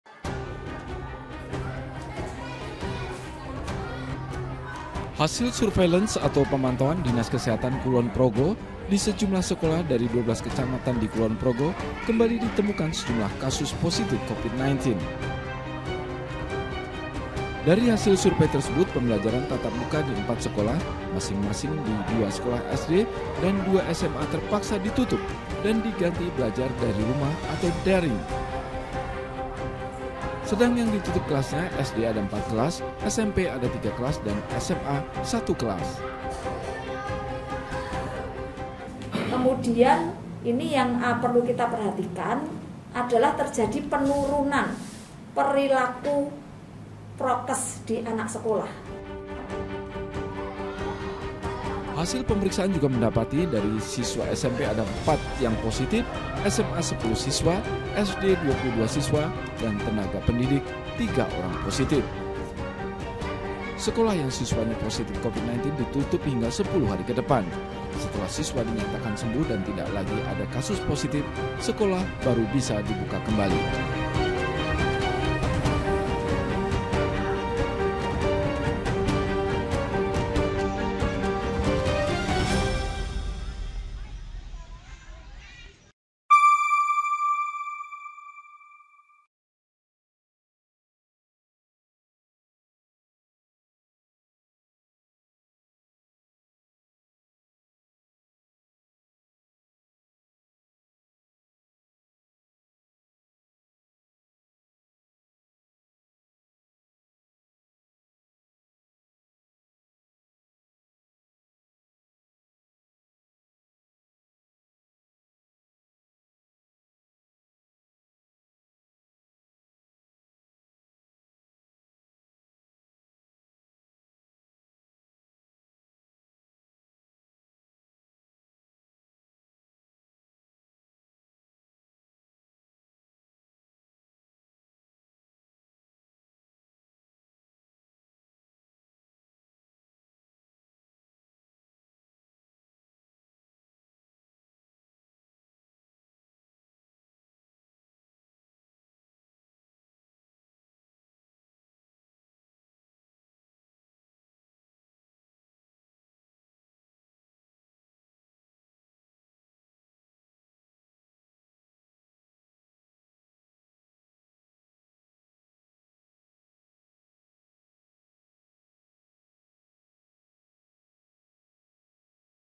4 Sekolah di Kulon Progo Ditutup dan KBM Kembali Daring, Begini Faktanya
Empat sekolah di Kulon Progo, Yogyakarta ditutup karena sejumlah siswanya positif covid-19, kegiatan KBM kembali dilakukan dengan daring. Peningkatan kasus covid-19, diduga akibat menurunnya tingkat disiplin terhadap prokes.

Android: inews.id/android
iPhone: inews.id/ios